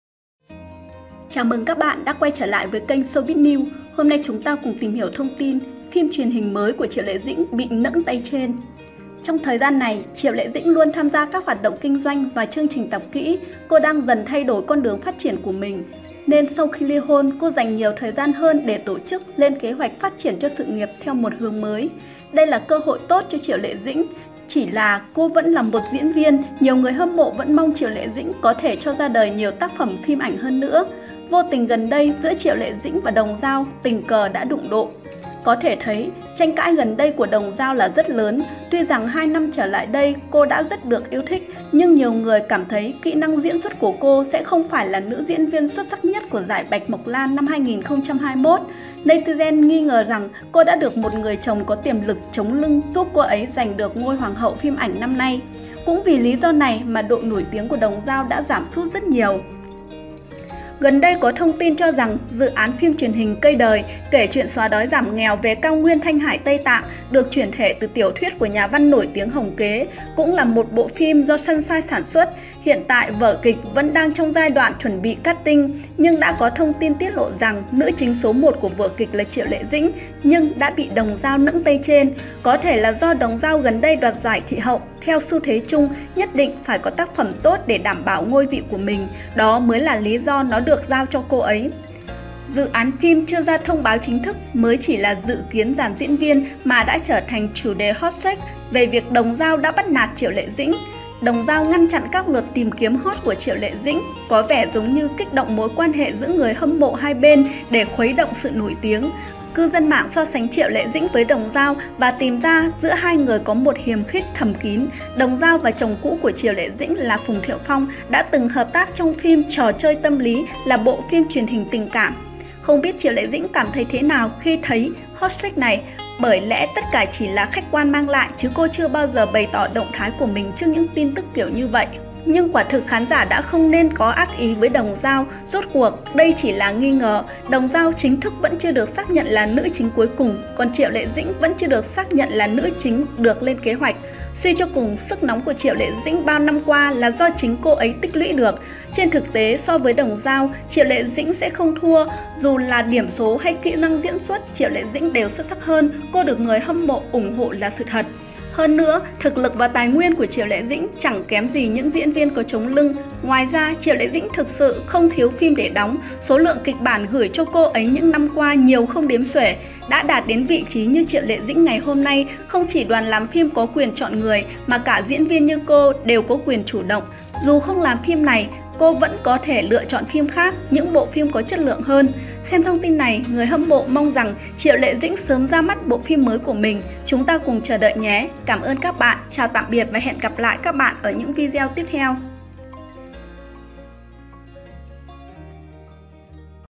Triệu Lệ Dĩnh bị Đồng Dao nẫng tay trên trong dự án phim mới
Gần đây có thông tin cho rằng, dự án phim truyền hình "Cây đời" kể chuyện xóa đói giảm nghèo nghèo về cao nguyên Thanh Hải-Tây Tạng, được chuyển thể từ tiểu thuyết của nhà văn nổi tiếng Hồng Kế. Cũng là một bộ phim do Sunshine sản xuất, hiện tại vở kịch vẫn đang trong giai đoạn chuẩn bị casting, nhưng đã có thông tin tiết lộ rằng nữ chính số một của vở kịch là Triệu Lệ Dĩnh, nhưng đã bị Đồng Dao nẫng tay trên. Có thể là do Đồng Dao gần đây đoạt giải thị hậu, theo xu thế chung, nhất định phải có tác phẩm tốt để đảm bảo cho ngôi vị, đó mới là lý do nó được cô ấy chọn.